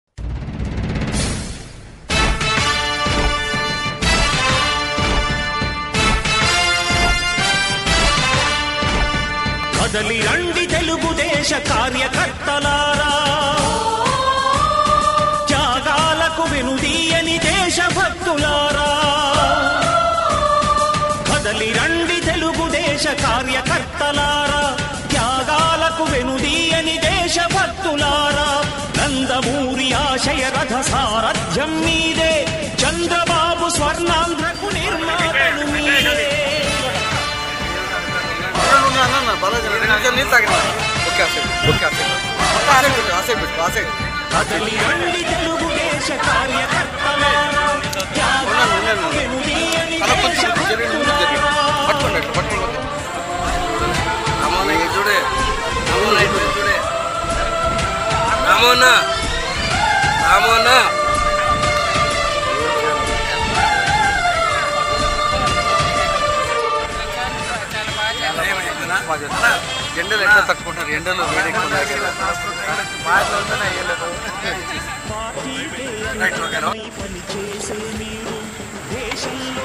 TDP Balakrishna Wife Vasundhara Devi Election Campaign In Hindupur | AP Election 2024 | DistodayNews
TDP MLA Nandamuri Balakrishna Wife Vasundhara Devi Election Campaign In Hindupur . AP Election 2024 . Distoday News